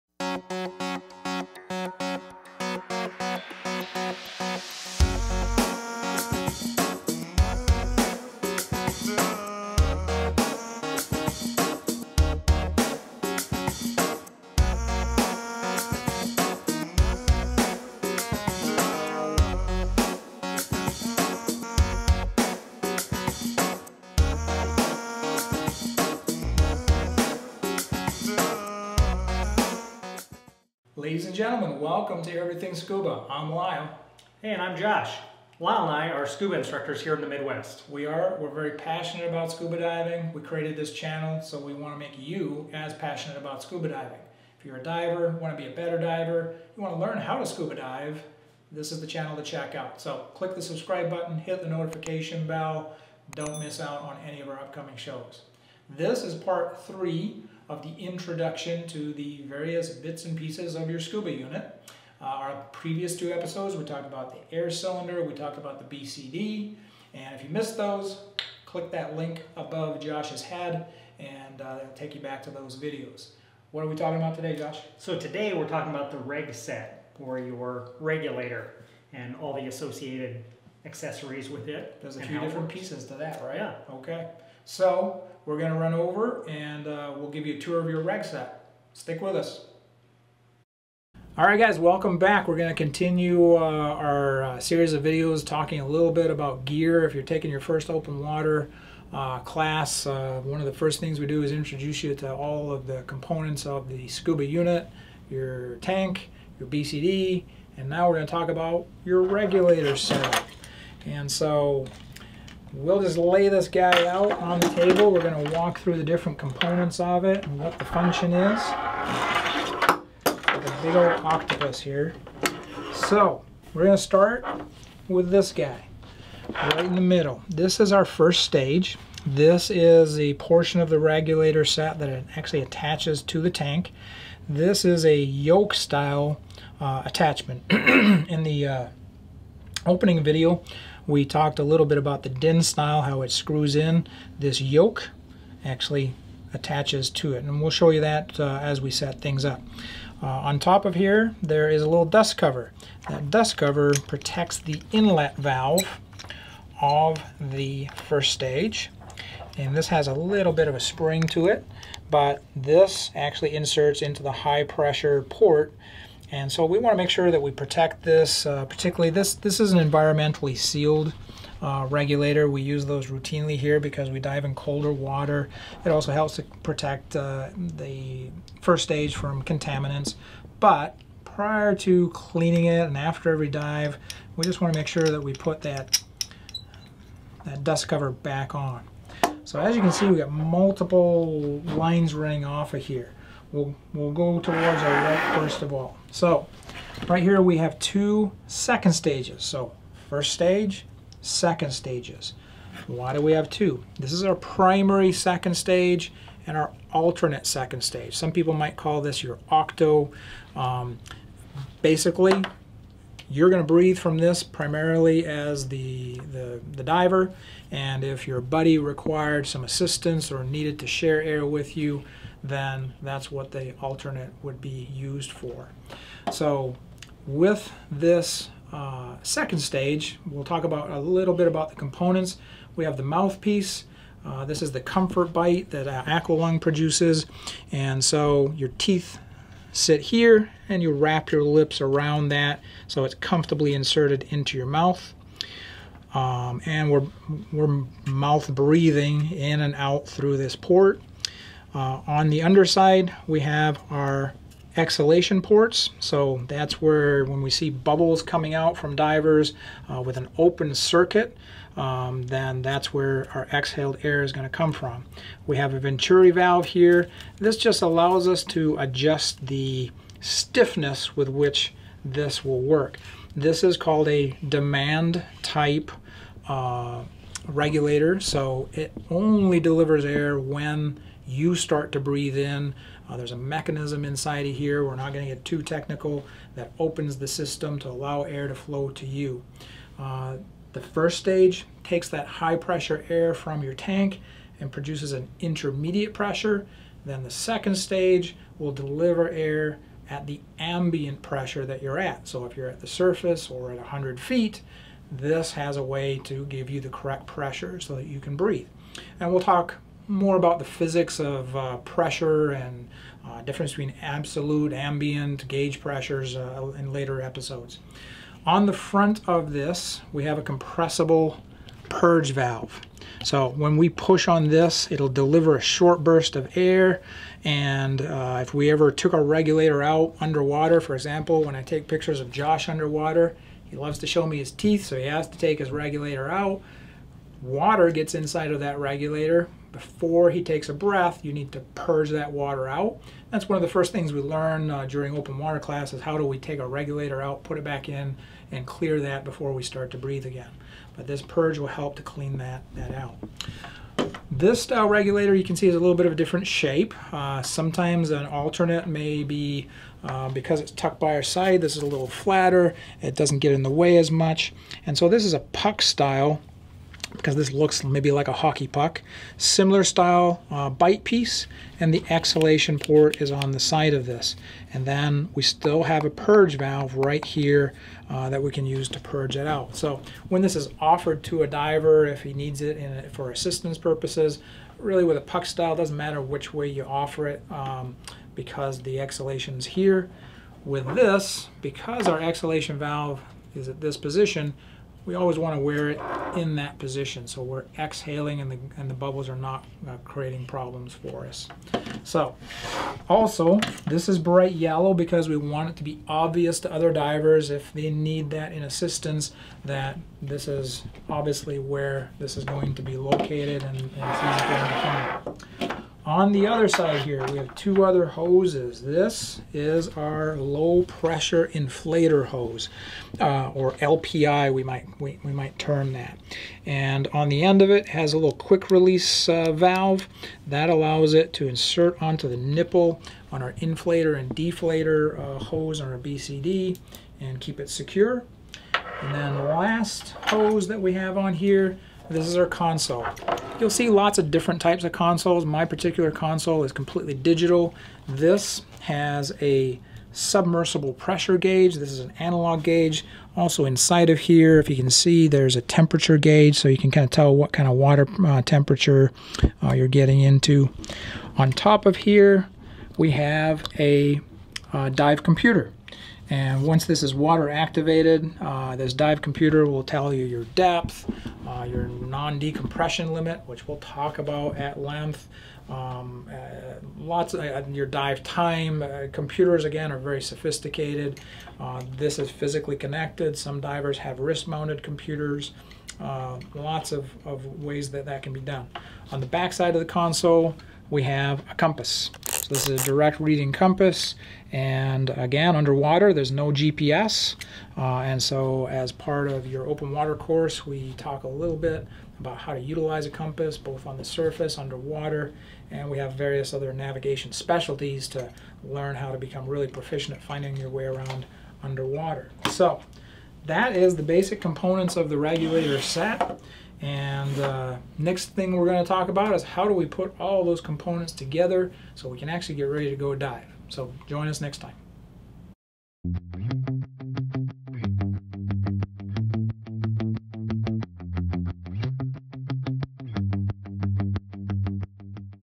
Scuba Regulator. It's component parts and functions.
As you begin your Open Water Diver course, the first items we introduce you tois the equipment you will need to know about and be familiar with to assemble your Scuba unit. Over the next 5 videos we will cover the three components of the scuba systems; tank/cylinder, BCD (Buoyancy Control Device), and Regulator set.

Lyell will introduce you to your regulator set, its functions and how to inspect it and get it ready for assembly.

Want to learn about Advance Open Water Diving? Watch this... "Top 5 tips for taking PADI Advanced Open Water Diver Certification "